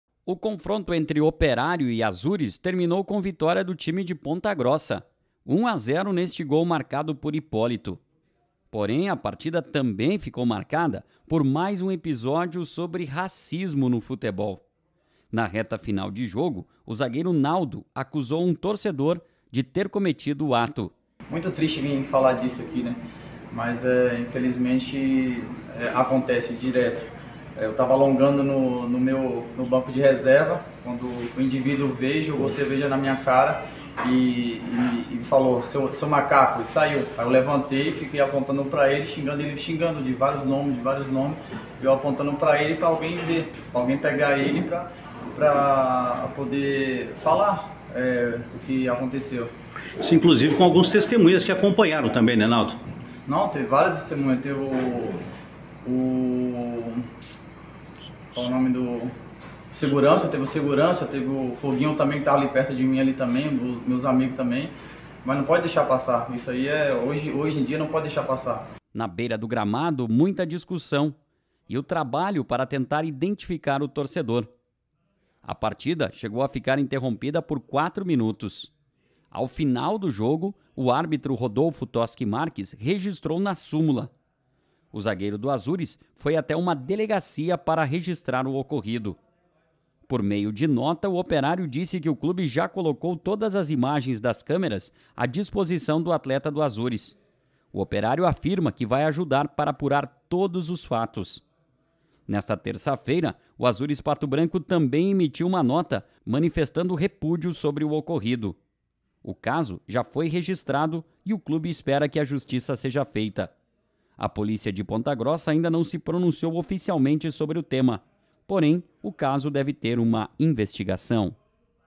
Repercute caso de racismo envolvendo jogo do Azuriz
Segurança: Repercute caso de racismo envolvendo jogo do Azuriz. O zagueiro Naldo registrou boletim de ocorrência.